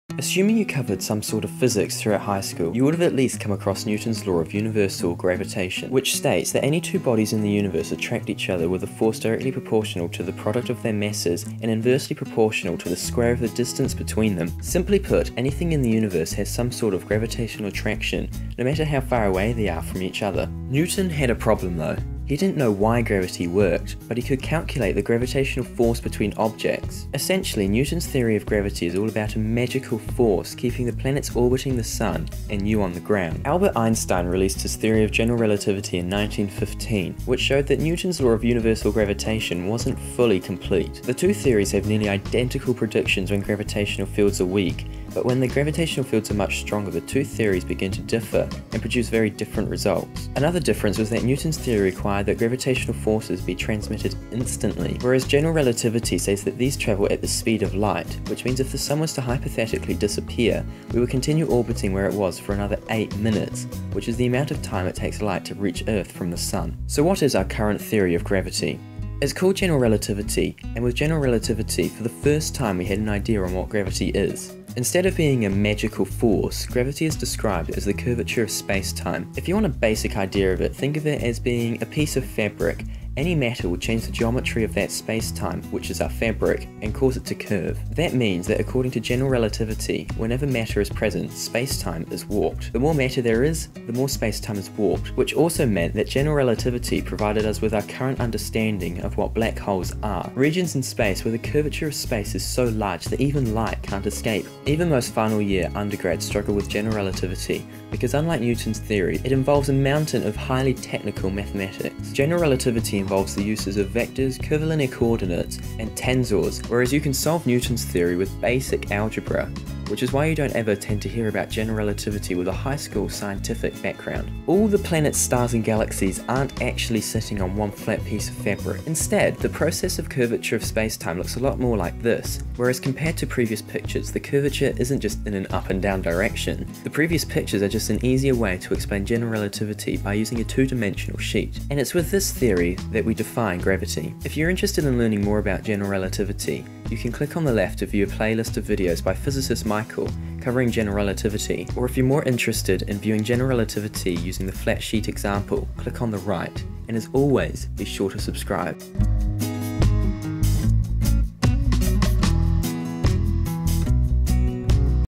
What Is Gravity? Newtonian Gravity vs General Relativity
What is gravity? It keeps you on the ground and the planets orbiting the sun. But do you really know what gravity is? If you haven't heard of General Relativity, chances are you don't know our current theory on what gravity is.